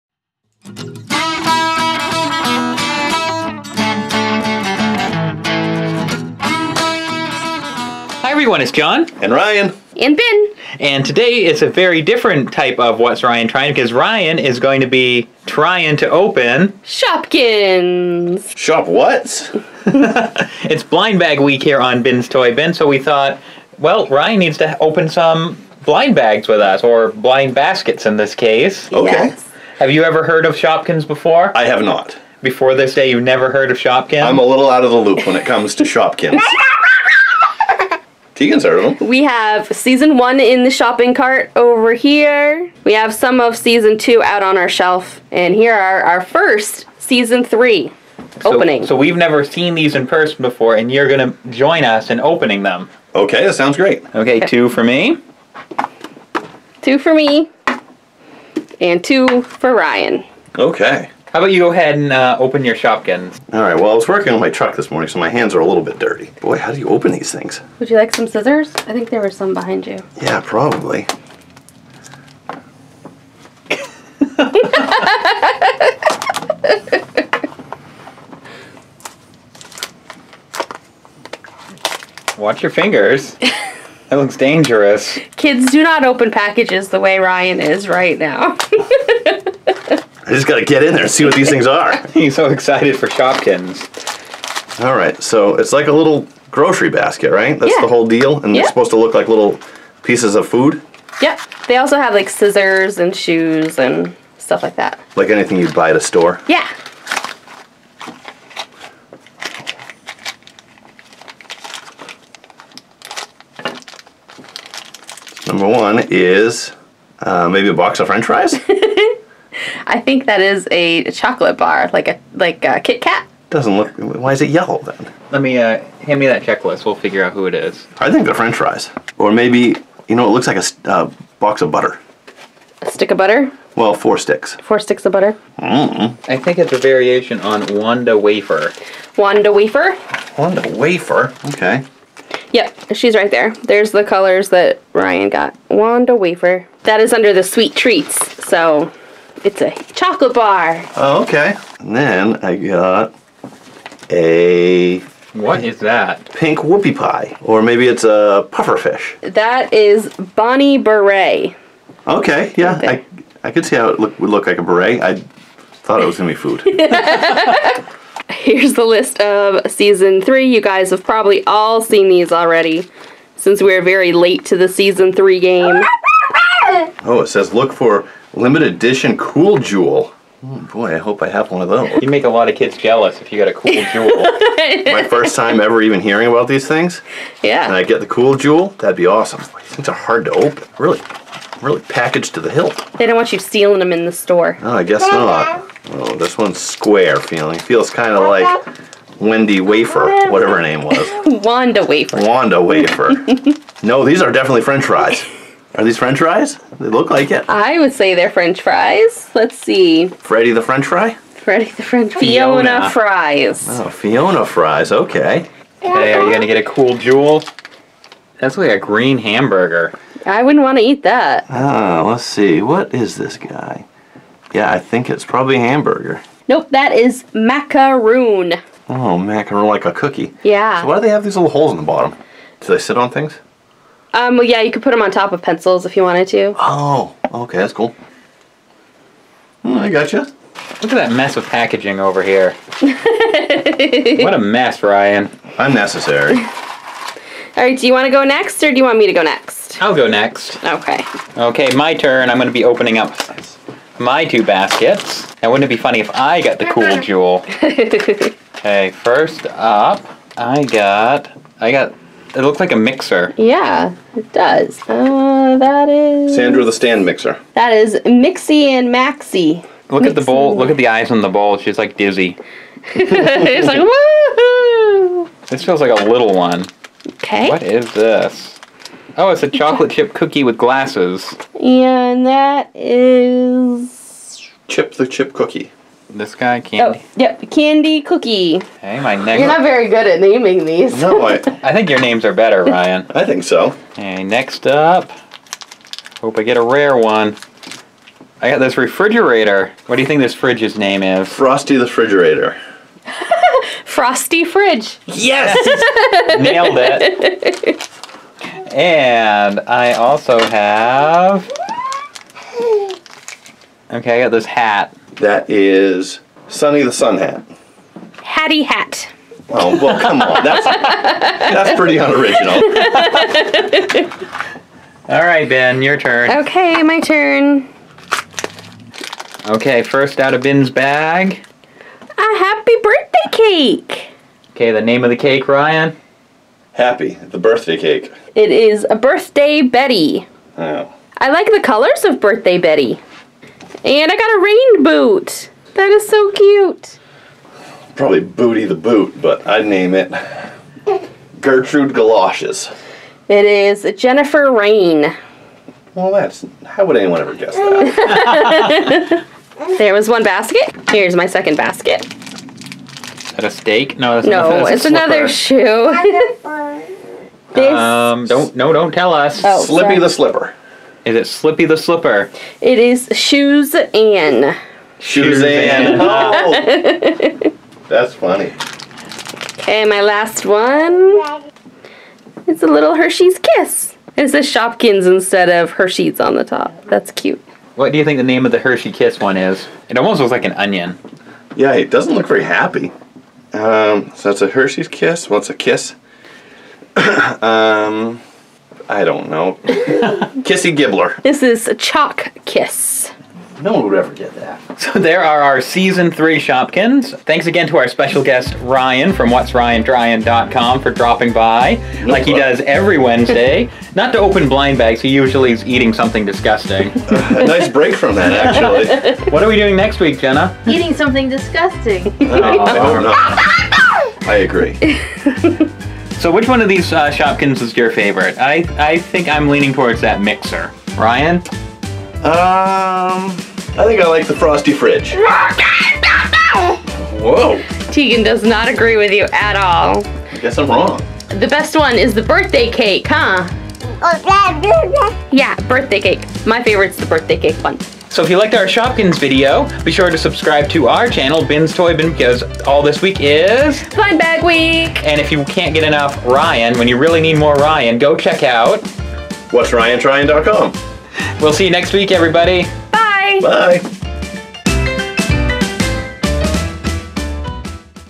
Blind Bag Week Day 4 with Shopkins!! What's Ryan Tryin joins Blind Bag Week
joins us for Blind Bag week and helps us open Season 3 Shopkins! Ryan has never heard of shopkins so this should be interesting.